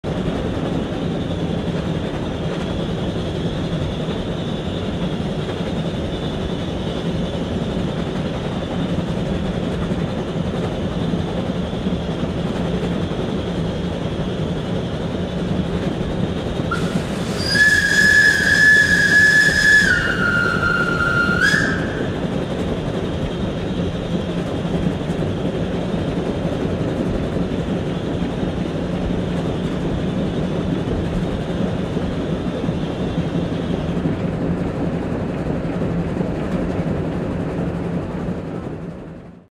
Höyryveturi: Vanha filmi 1950 luku
Höyryveturi puksuttaa. Höyryjuna: Vanha filmi 1950 luku.
Kesämaisemia junan ikkunasta. Vehkosillan seisake. Tuotanto / Tuotanto: Fennada-Filmi Junior. Alkuperäinen filmi / Alkuperäinen elokuvan nimi: VR Henkilöliikenne. Valmistumisaika / Valmistusaika: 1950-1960-luku / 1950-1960's. Mykkäfilmi / Silent film. Värifilmi / Värikalvo. Lisenssi / Lisenssi: Creative Commons Nimeä / Nimeä 3.0
YleArkisto
13. tammikuuta 2017
Höyryveturi kulkee, vauhti kihtyy. Vihellyksiä. Sisä.
Äänitetty / Rec: Analoginen nauha, Nagra / Analog tape, Nagra
Paikka / Paikka: Suomi / Suomi / Pieksämäki